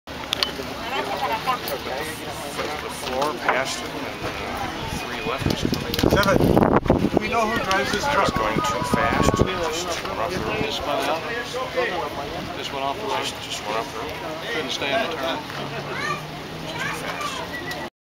2009 La Carrera Panamericana Day 0 Hualtuco Clyde Morter talks about Lucky Taco Oldsmobile crash
Clyde Morter, #421 Original Panam Oldsmobile "The Lucky Taco" talks about his offroad excursion on Day 0 near Hualtuco, Oaxaca, Mexico.